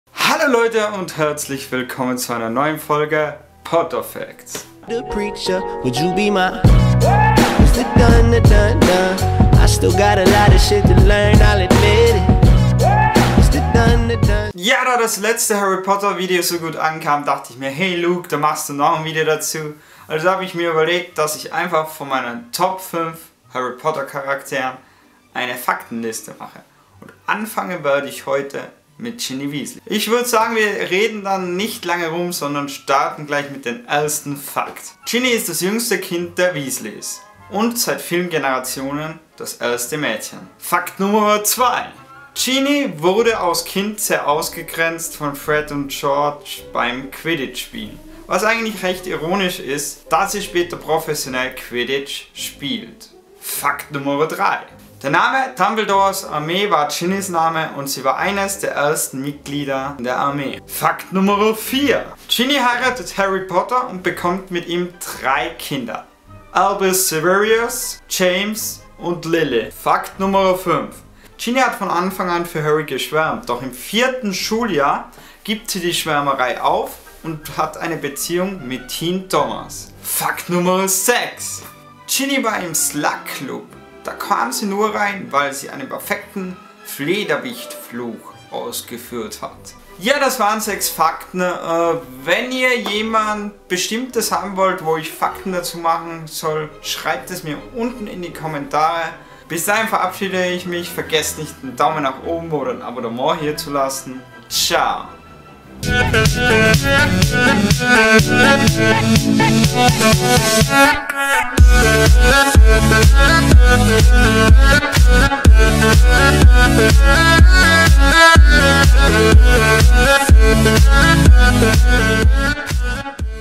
6 Fakten zu Ginnny Weasley Potterfacts 2
Eine neue Folge Potterfacts ist am Start Heute gibt es 6 Fakten zu Ginnny Weasley. Viel Spaß  und Kommentieren nicht vergessen.

► MEIN EQUIPMENT ◄
* Kamera: Canon 7D
* Stativ: Hähnnel
* Objektiv: Sigma 10-20 mm Weitwinkel
* Beleuchtung:Walimex Softbox